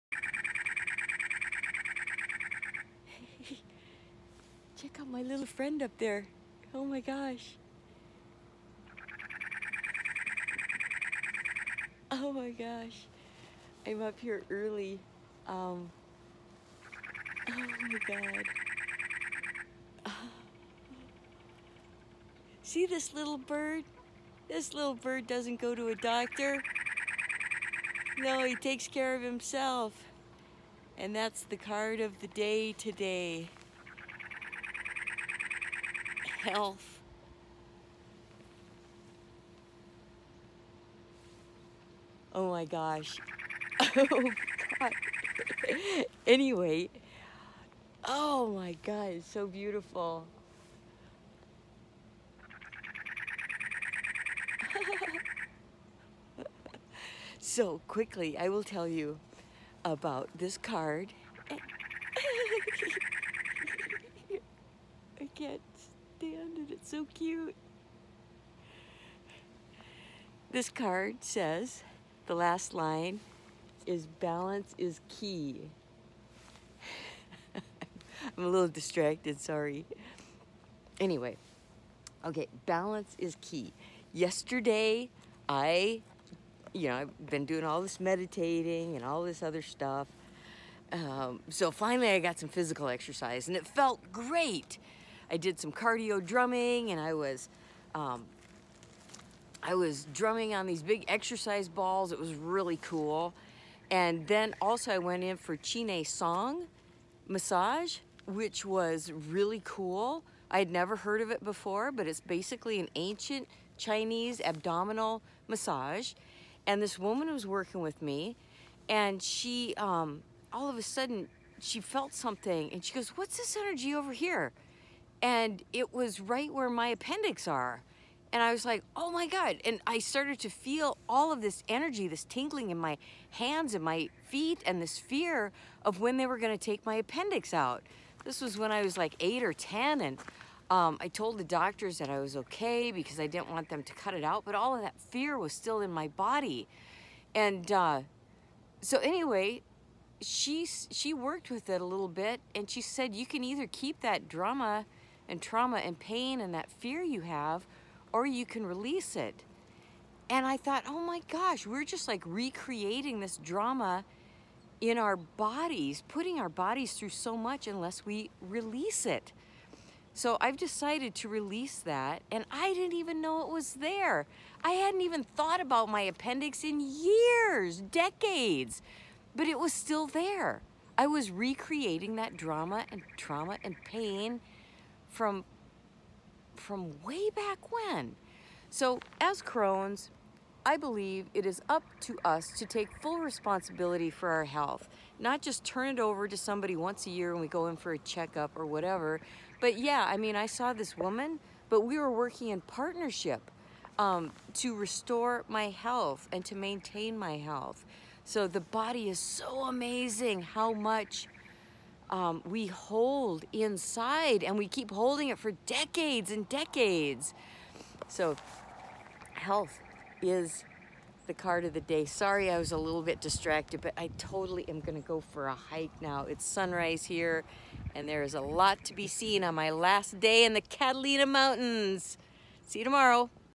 Claiming my Cronedom ~ Health
A little bird steals the show in this one! I talk about the responsibility we have as Crones to our bodies. A bit about my first experience with Chi Nei Tsang